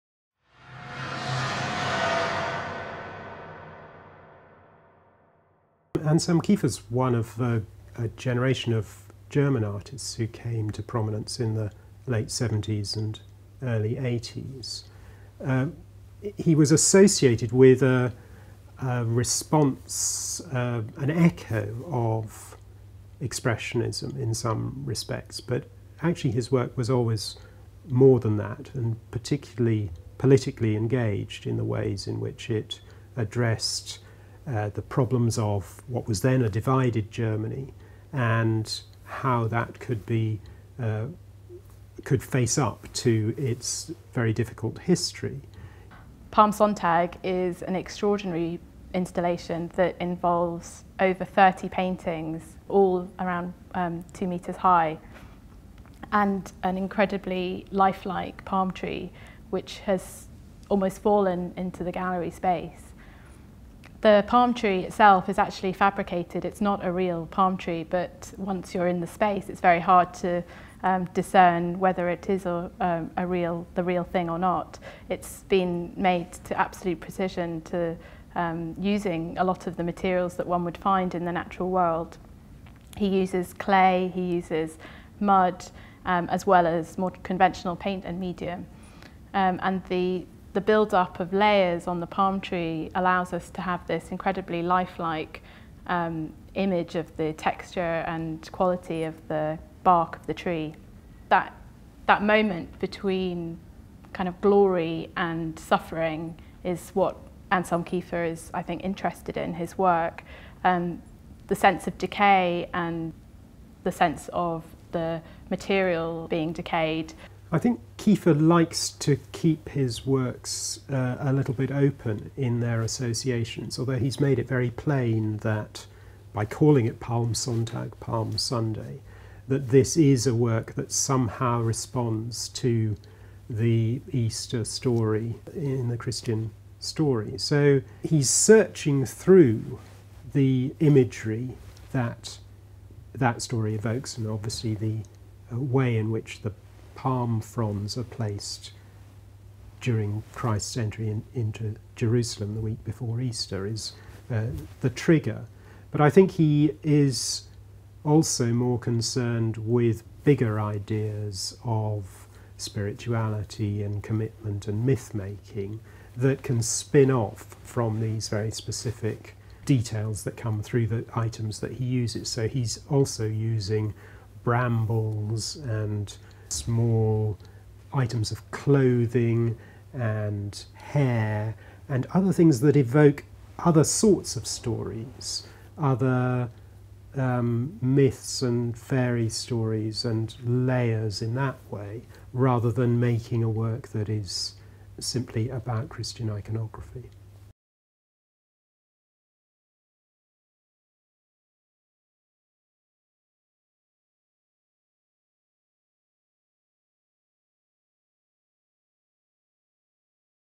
ARTIST ROOMS: Anselm Kiefer
The newly expanded Mostyn in Llandudno, which opened following major building work in 2010, will provide a dramatic backdrop for the powerful work of German artist Anselm Kiefer. This will be the first time Kiefer's work has been shown in Wales. As well as presenting a range of the artist's work held in ARTIST ROOMS, such as the monumental Palm Sunday (Palmsonntag) 2006 the show will explore themes in Kiefer's work that have a resonance with the culture of Wales, particularly in relation to notions of the weight of mythology and history, and their deep links with the landscape.